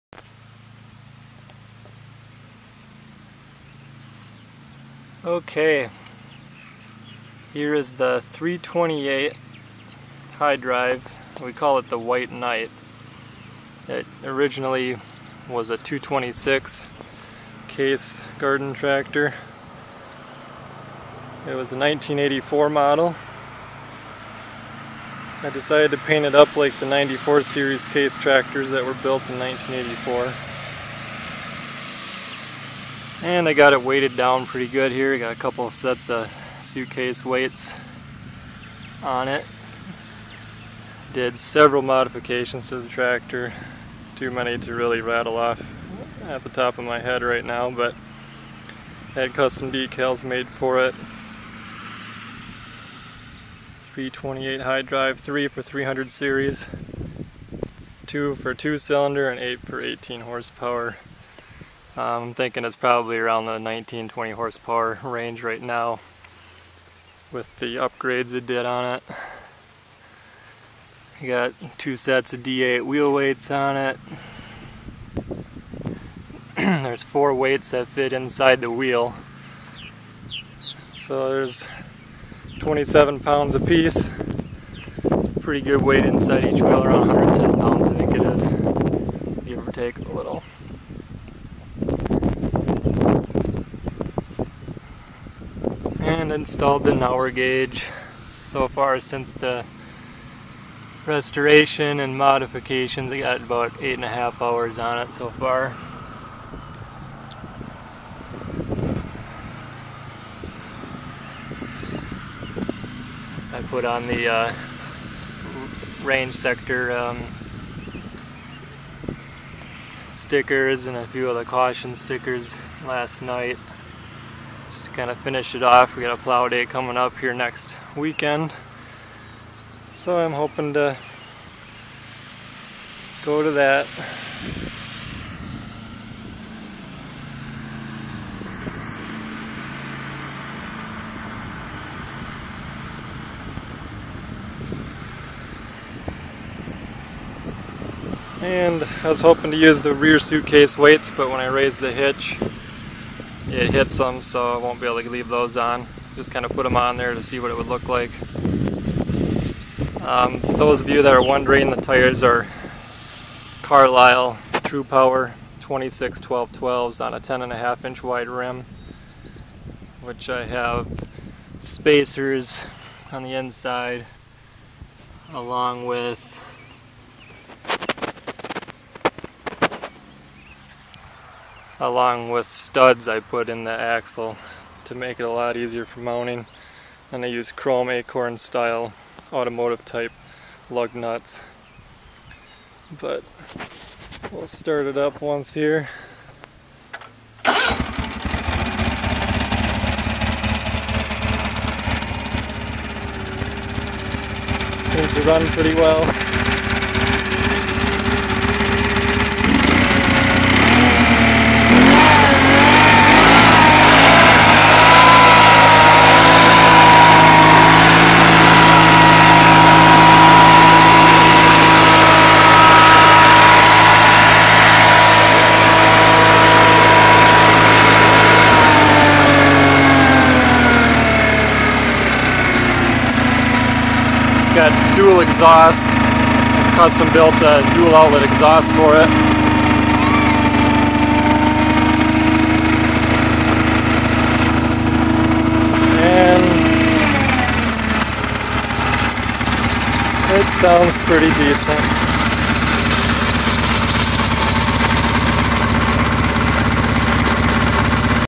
CASE White Knight Demonstrator 328 Garden Tractor.
I took a little clip of the WK "White Knight" with me doing a quick walk around and engine start. At the end my son needed my attention so I had to cut it short. lol.
The weights on the back interfere's with the hitch so they are just mounted for your enjoyment.
This project was a dream I've had for many years to build a custom Garden Tractor like this and it's been rewarding to finish it and now to enjoy the fruits of my labor.
A BIG Thank You to all those in the Case/Ingersoll GT community for your support and encouragement through out the rebuild. You know who you are. ;)
CasecoltIngersolldotcom